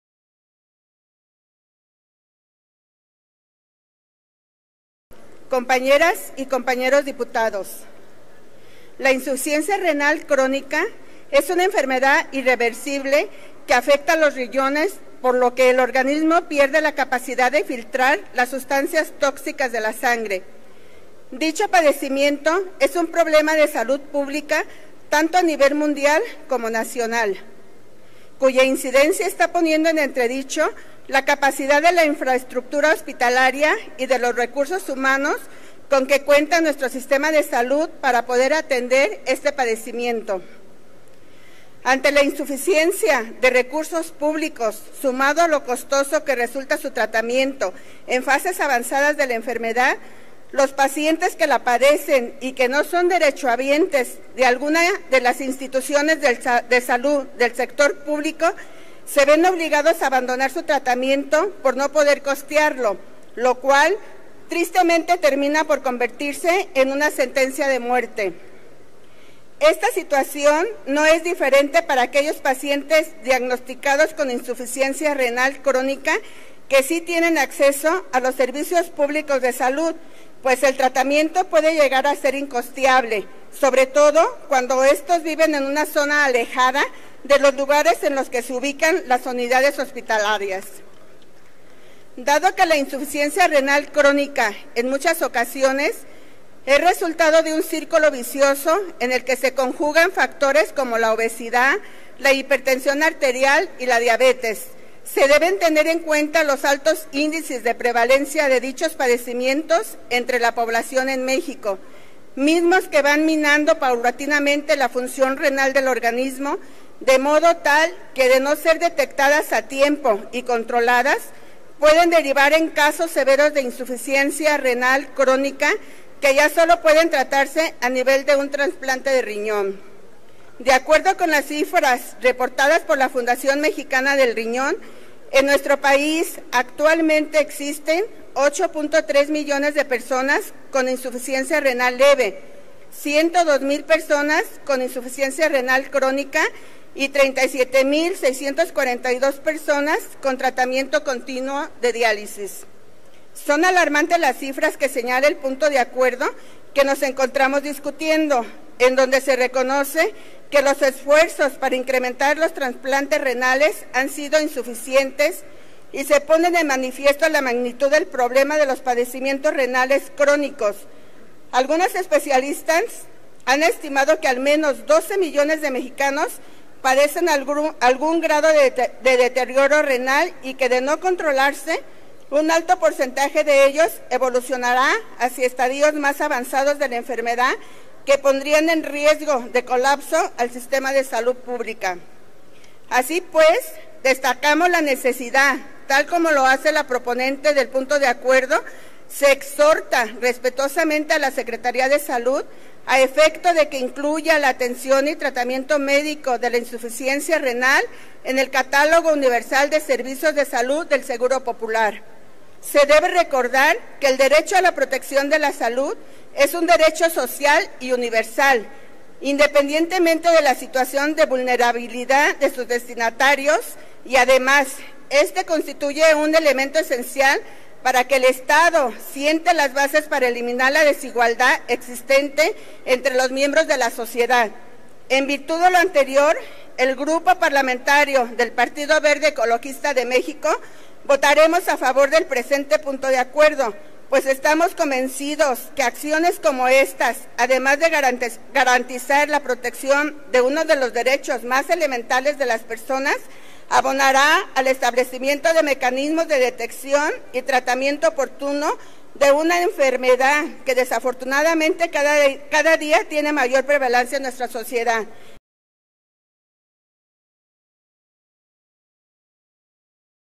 Participación de la Dip. Rosa Álvarez - Garantizar el derecho a la salud de las personas
La Dip. Rosa Alicia Álvarez Piñones (PVEM) precisa que la insuficiencia renal es una enfermedad irreversible que afecta a los riñones. Asegura que se trata de un problema de salud pública a nivel nacional e internacional.

Indica los factores que inciden en dichos padecimientos y precisa la magnitud de los problemas renales crónicos, así como los porcentajes de incidencia. Finalmente, se pronuncia en pro de punto de acuerdo para garantizar el derecho a la salud de las personas.